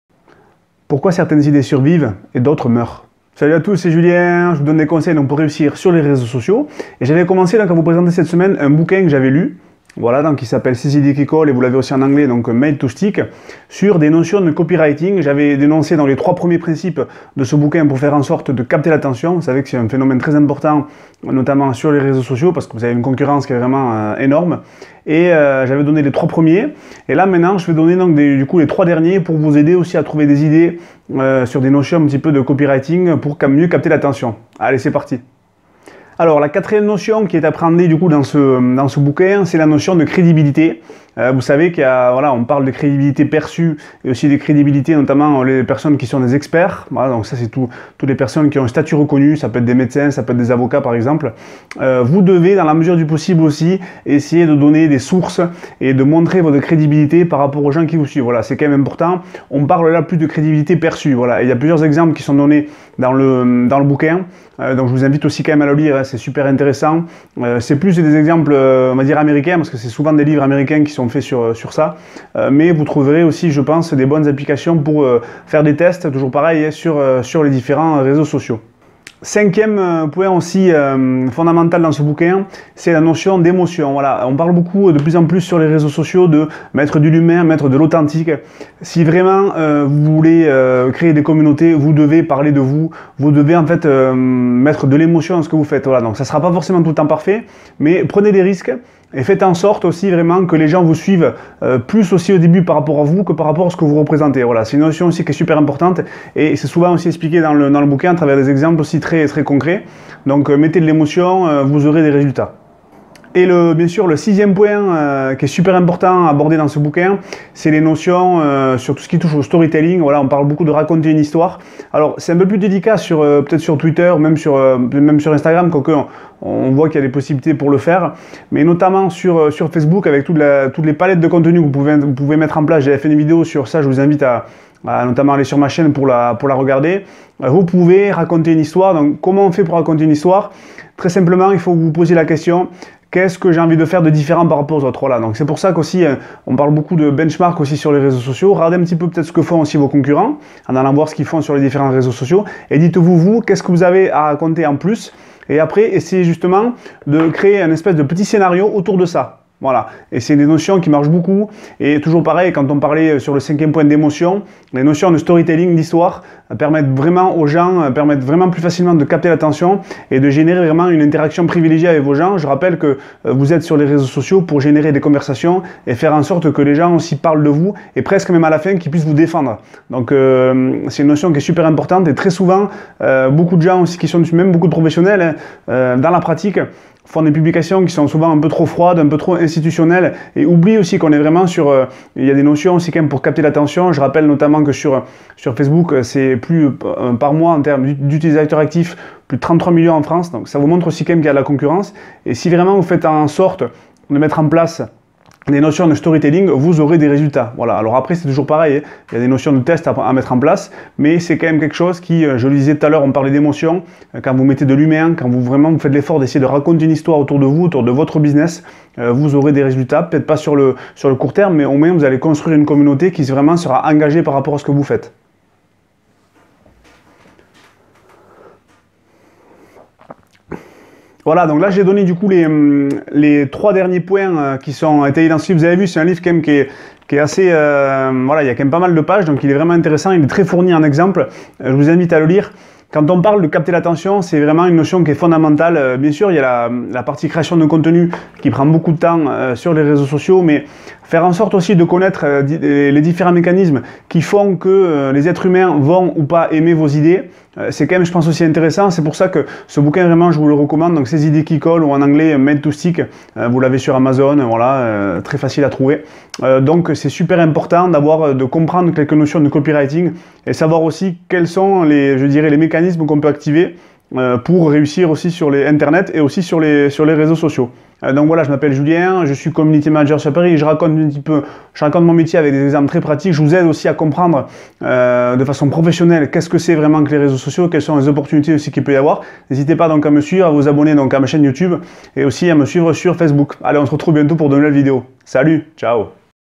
6 techniques pour capter l'attention sur les réseaux sociaux 2/2
Le livre s'appelle "Ces idées qui collent" en français

Qui est Julien Pibourret ?